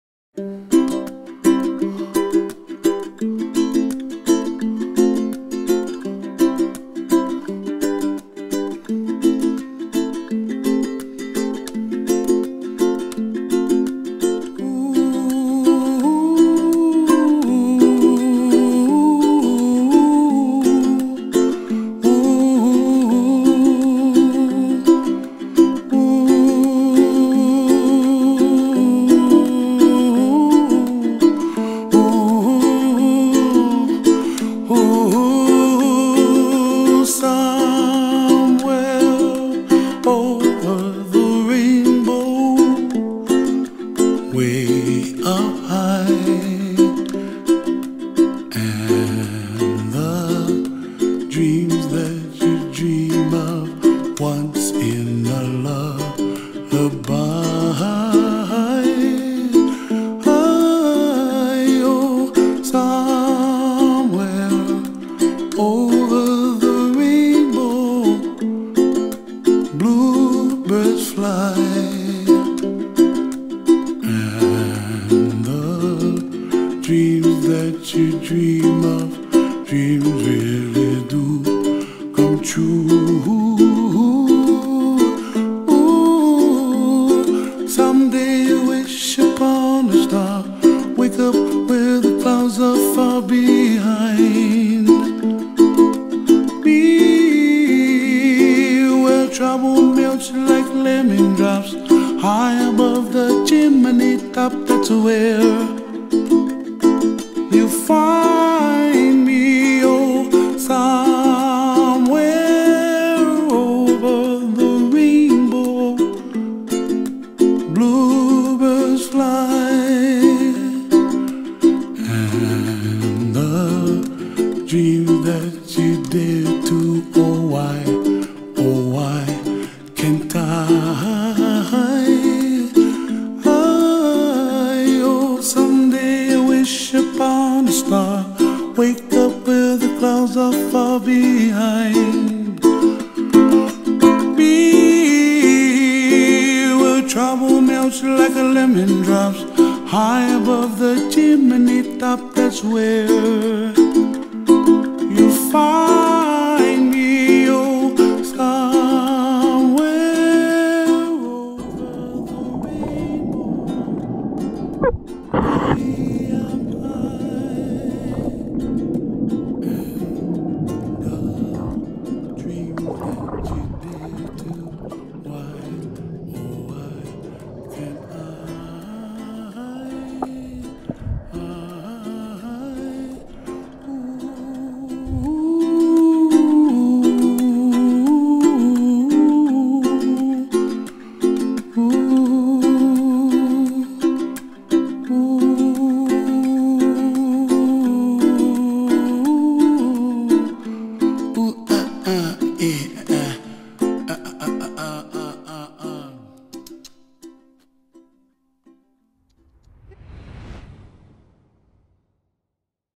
Carnival sunshine cruise to Nassau Bahamas
Half moon, cay, pearl island, snorkeling excursion. ￼